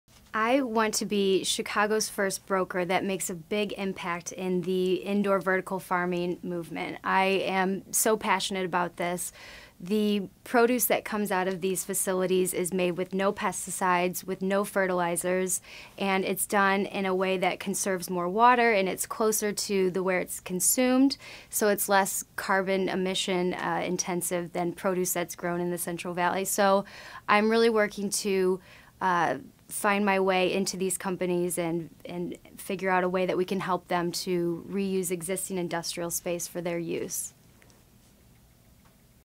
20 in their 20s: Kelly Gray
Within the grit of industrial real estate, Kelly Gray is the rare broker who begins the day with yoga and ends it with meditation.